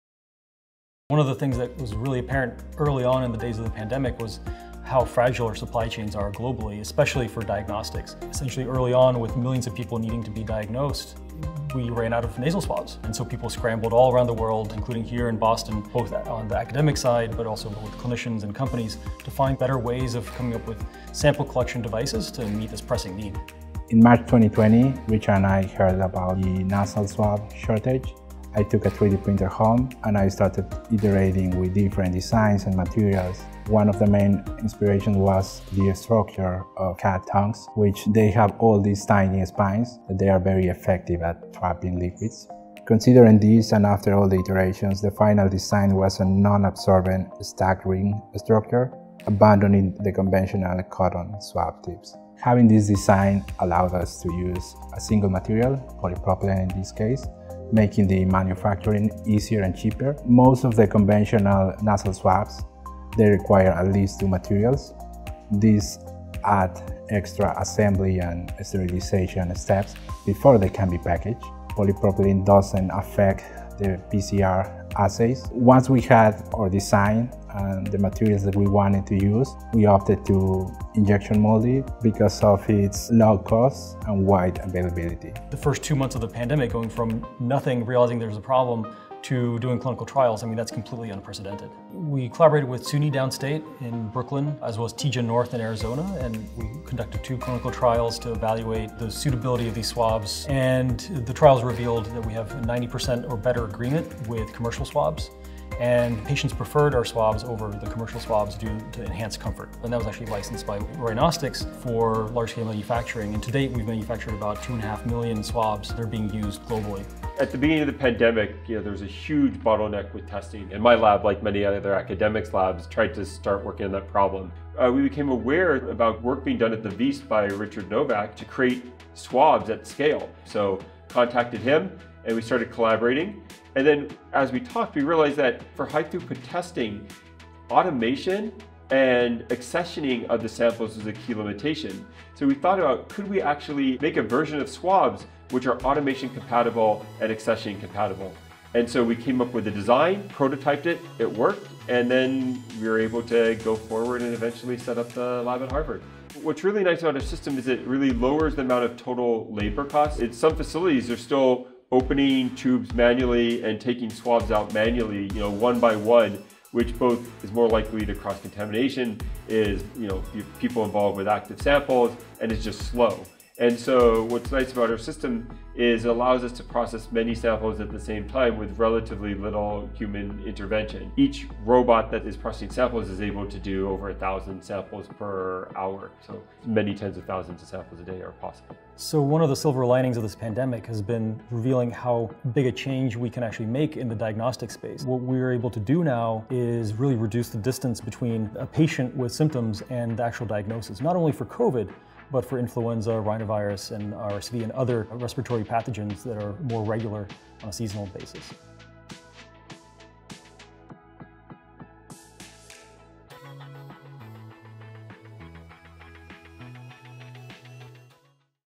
Beating Back the Coronavirus: Nasal Swabs
Early in the COVID-19 pandemic, Nasopharyngeal swabs or nasal swabs, used to collect mucus samples to test for the SARS-CoV-2 virus, were in short supply. This created a bottleneck in diagnostics, hampering our ability to control the pandemic. To respond to this need, an interdisciplinary team at the Wyss Institute and Harvard Medical School collaborated to create a new nasal swab design from only one material with 90% or better agreement with existing swabs, and the ability to be used in much faster automated analytical systems.